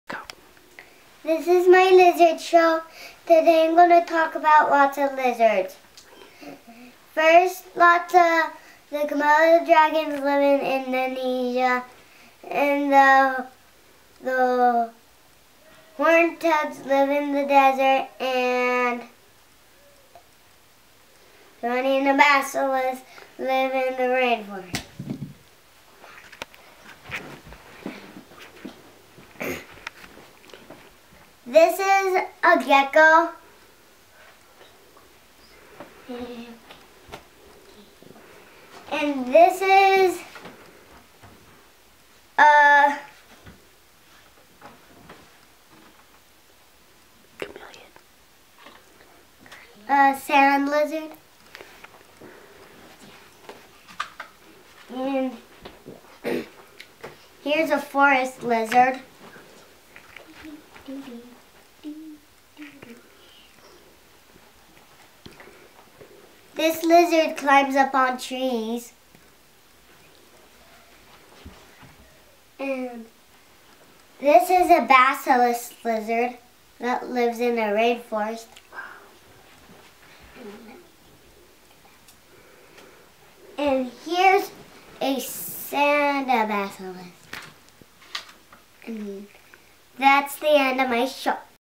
Collin's Lizard Video #1
Collin, age 6, feels there are not enough videos about lizards on youtube. He decided to make his own. He didn't rehearse, but you can tell how happy is talking about the lizards.